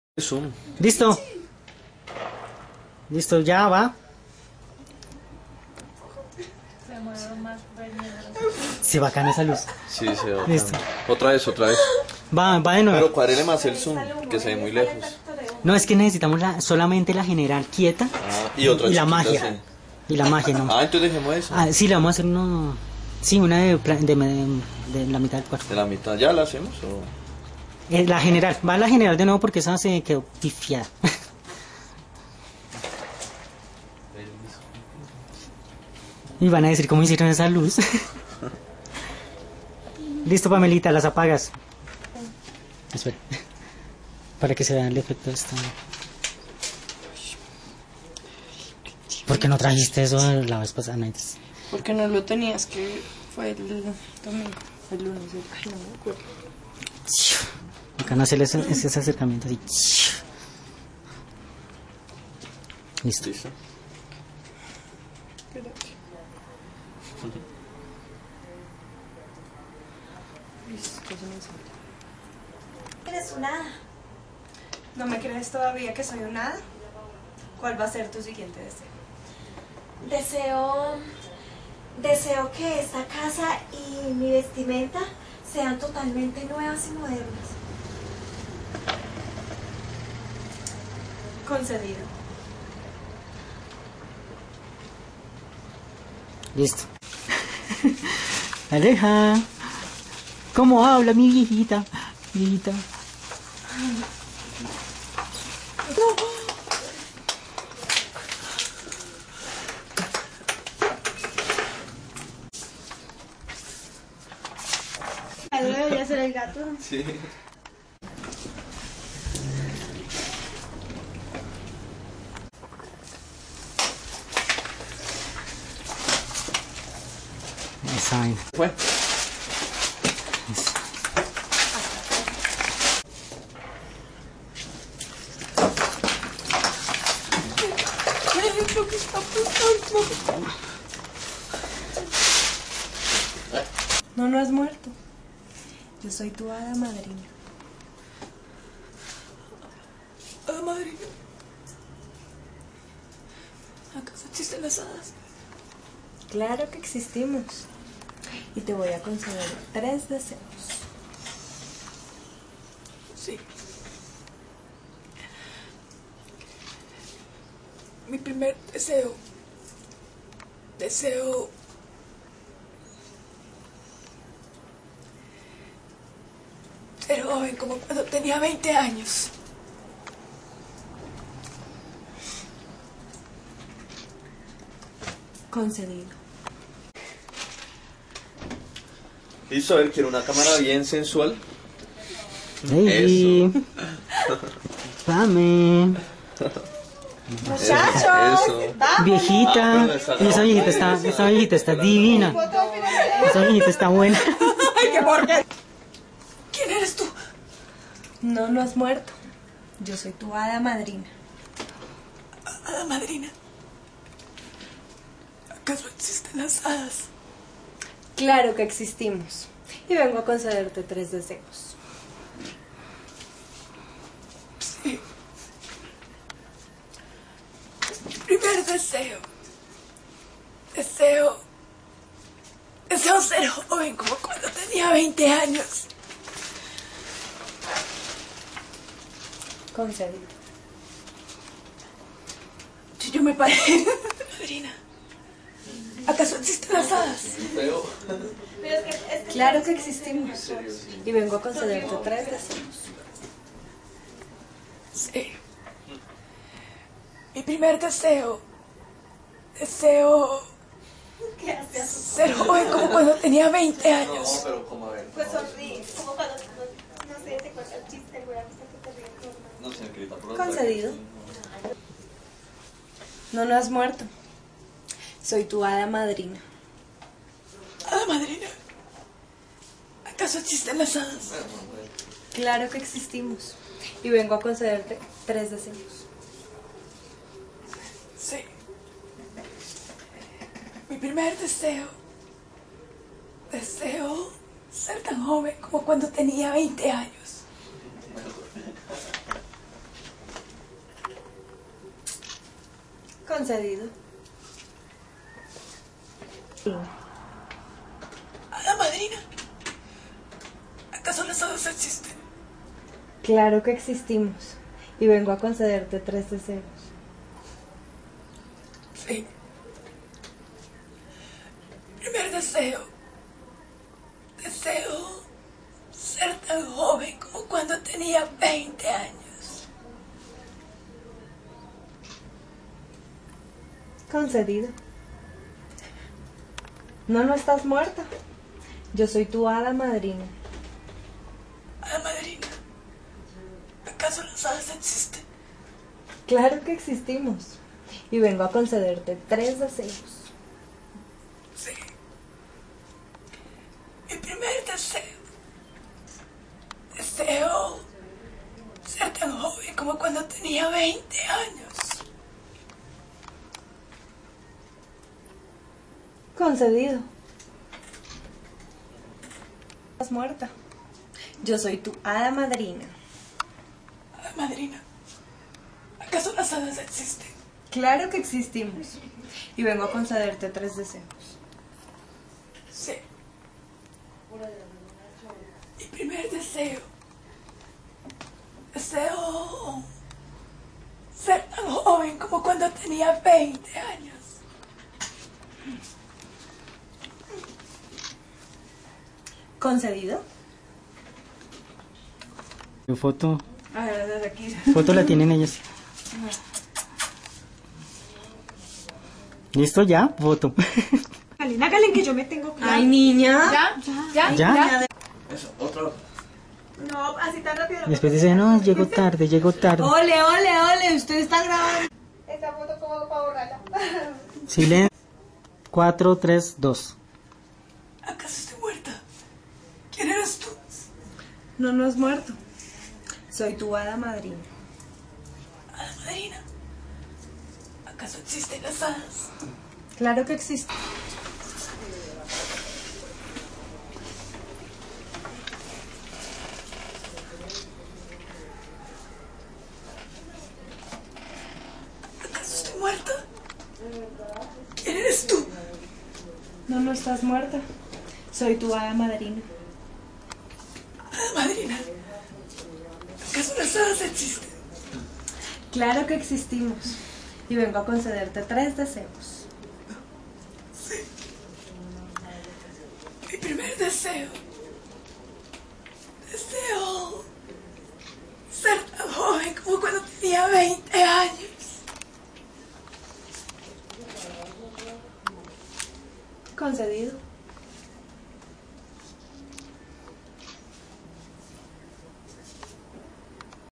Documental MAKING Off Parte 1: "CONCEDIDO" Por: RICARDO VELASCO
Material INÈDITO (Ensayando SECUENCIAS)

SINOPSIS: . Al aparecerse un HADA MADRINA le concede tres deseos a una viejecita y entre los tres deseos le concede convertir a su GATO en príncipe este sorprende a la mujer aclarándole (reclamándole) lo que hizo con él, en su vida anterior cuando era GATO.

PERSONAJES: VIEJECITA, HADA, MUJER JOVEN, PRINCIPE GATO.

REPARTO: ALEJANDRA VILLAMIZAR,PAM DIAZ MARTINEZ,LEYDI NATALI GALLEGO Y JOHN F. MONTOYA.
PRODUCTOR DESIGNADO: ALEJANDRO ACOSTA CIFUENTES, MÚSICA: RICARDO VELASCO POSTPRODUCCIÒN: HERBERT SALAZAR, VESTUARIO MAQUILLAJE Y UTILERIA: CEDELINA SOSA,LEYDI NATALI GALLEGO,JORGE RAMIREZ, LORENA MONTERO,RICARDO VELASCO DOBLE: LORENA MONTERO
PRODUCTORES ASOCIADOS:ALEJANDRA VILLAMIZAR,PAM DIAZ MARTINEZ,LEYDI NATALI GALLEGO, JOHN F. MONTOYA. RICARDO VELASCO
PRODUCCIÒN, GUIÒN Y DIRECCIÒN: RICARDO VELASCO. BOGOTÀ COLOMBIA 2010
Lugar: IMAGIMAGICO PRODUCCIONES